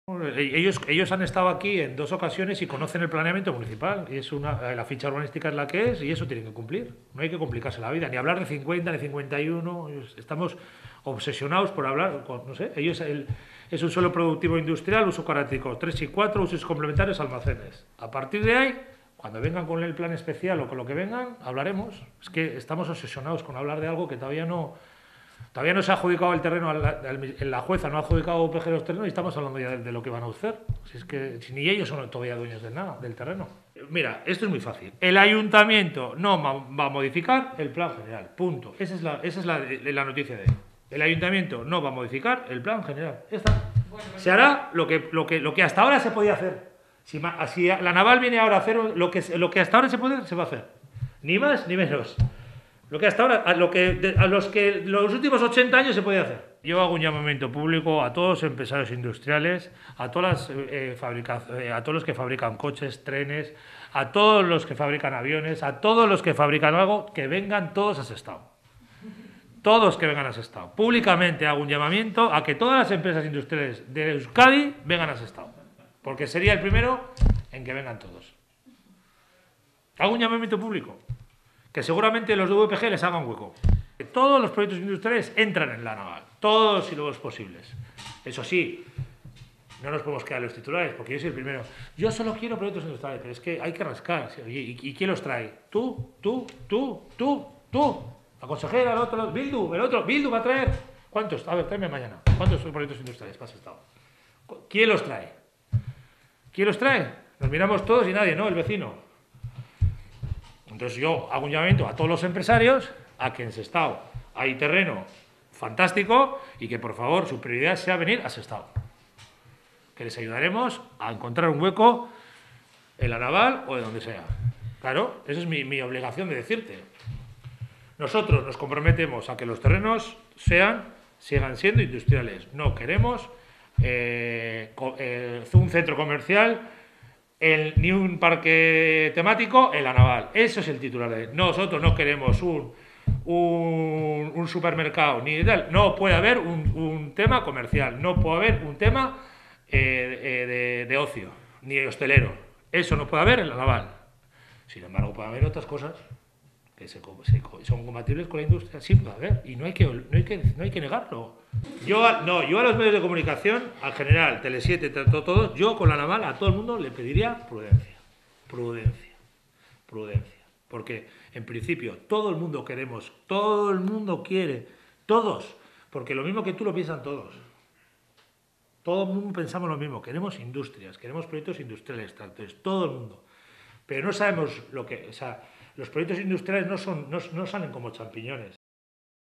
El alcalde de Sestao habla claro sobre el destino industrial de los suelos de La Naval
El pleno ha aprobado hoy por unanimidad una declaración institucional en este sentido.

Todos los partidos políticos se comprometen a que las nuevas actividades económicas que se asienten en estos terrenos se ajusten al uso establecido en el Plan General de Ordenación Urbana del municipio, que los cataloga como “Suelo Productivo Industrial, con oficinas y almacenes complementarios”.

En respuesta a las preguntas de TELE7, el alcalde, Josu Bergara, ha sido tajante: